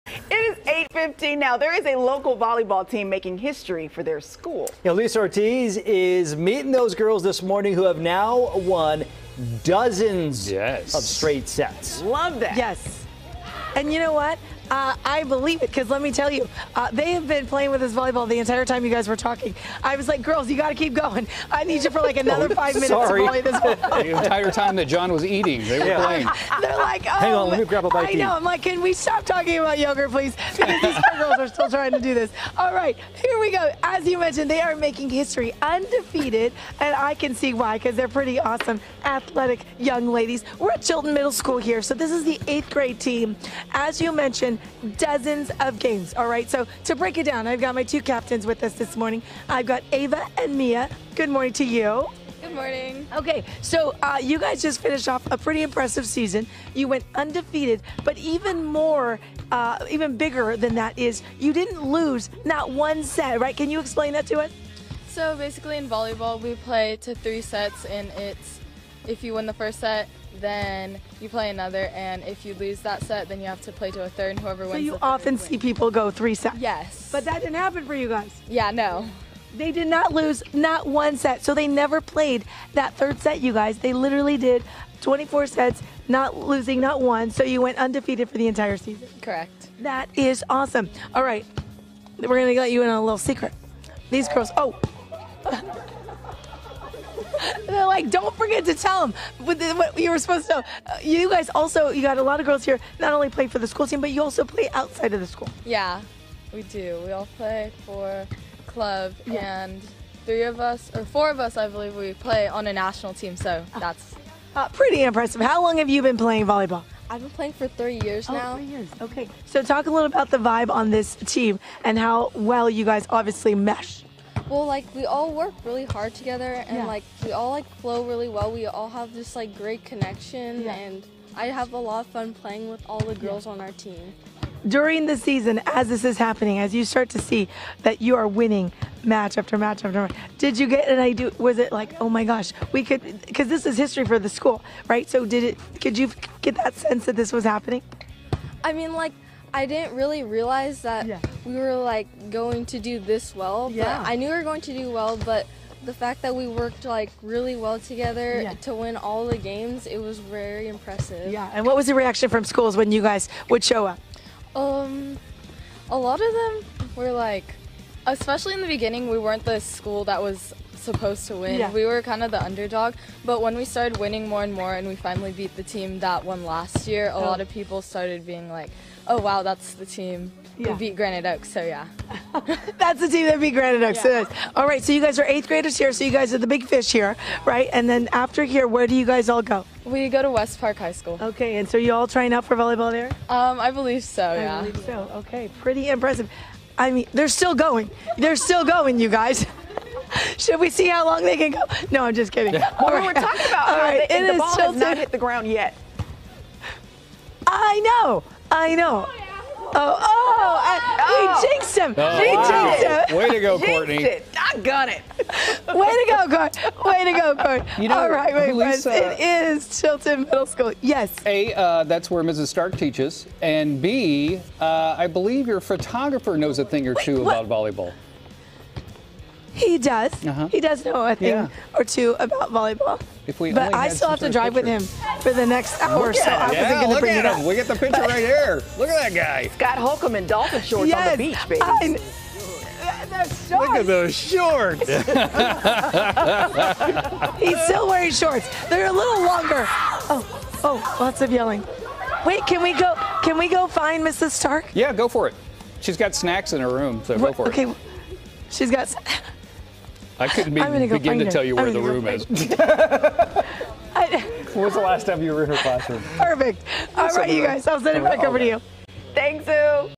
Volleyball Champs!
Julissa Ortiz is in Roseville at Chilton Middle School, meeting their undefeated girl's volleyball team!